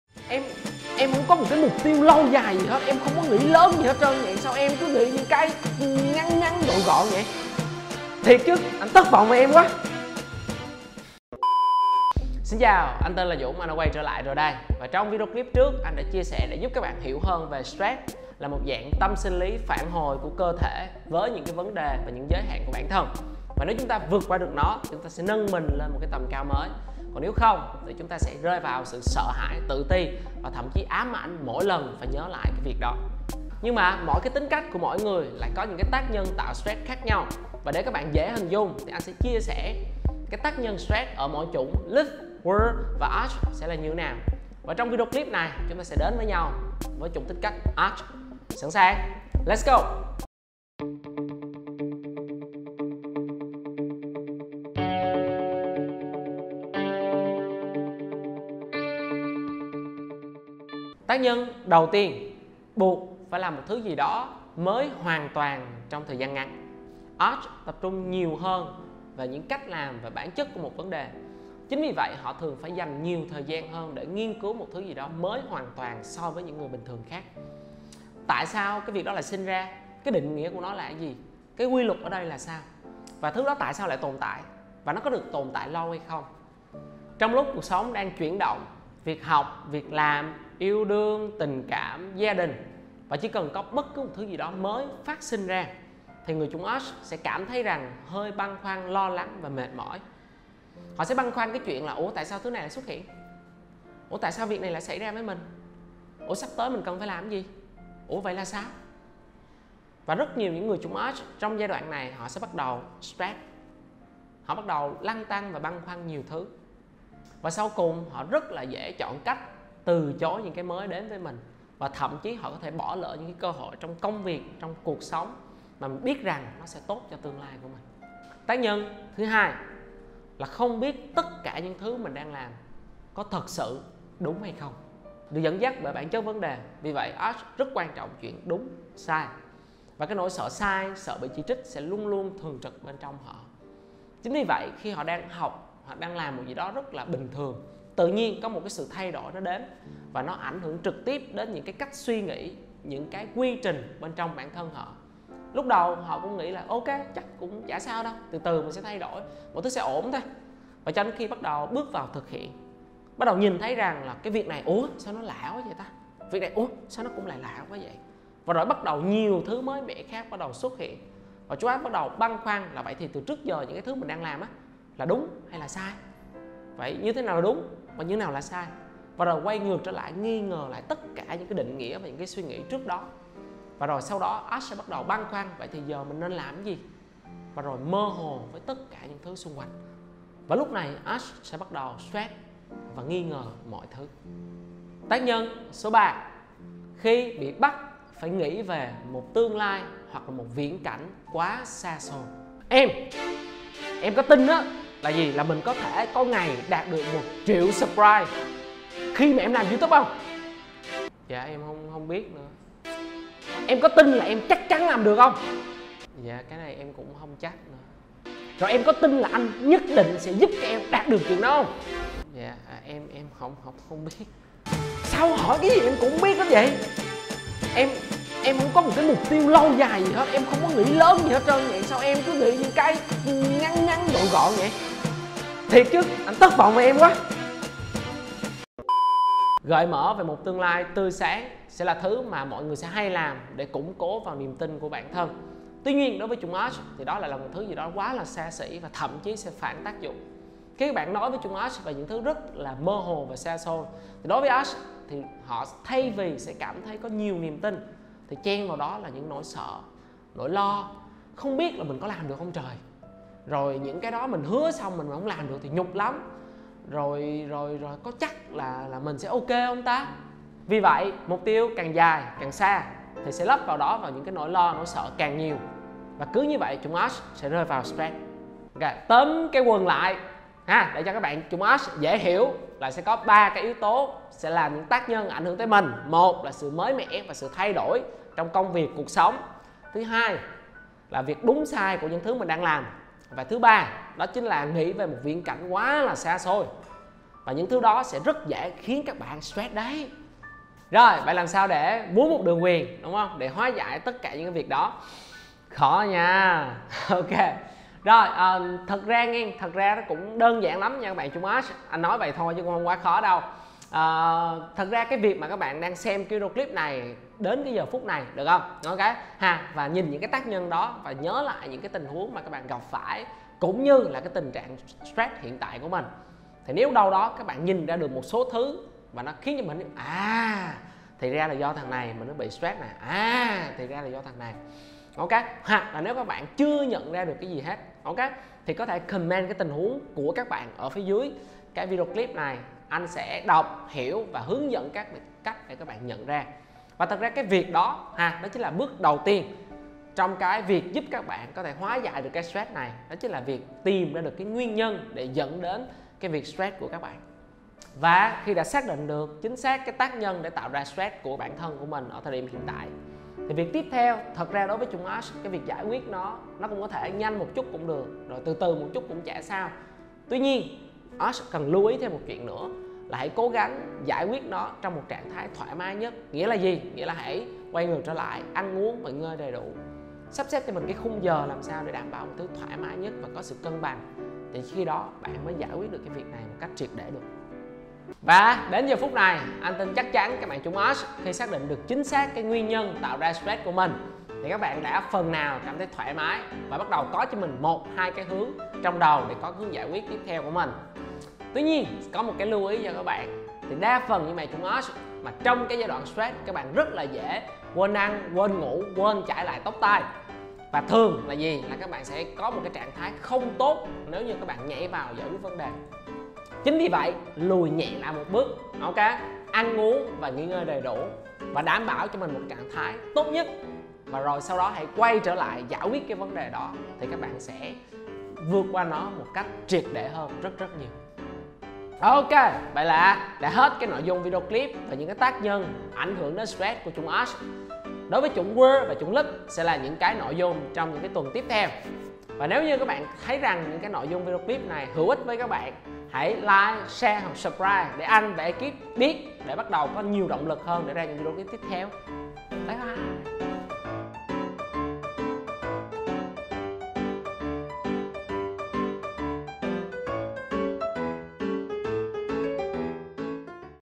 Chủng vân tay ARCH thường STRESS vì điều gì? |Hiểu bản thân với Sinh Trắc Vân Tay INgô Thanh Dũng
Chào các bạn! Video tuần trước anh đã chia sẻ chung những sự thật về stress mà ai cũng cần phải biết. Tuần này, chúng ta sẽ đi sâu hơn về Stress ở chủng vân tay Arch - chủng vân tay hiếm nhất thế giới và cách để giúp Arch vượt qua stress nhé!

Ngoài Sinh Trắc Vân Tay thì sắp tới anh cũng sẽ chia sẻ về những kiến thức, kỹ năng mềm bổ ích về tâm lý và đời sống.
*Tìm hiểu thêm về anh Ngô Thanh Dũng - chuyên viên tư vấn về công nghệ Sinh trắc học vân tay tại: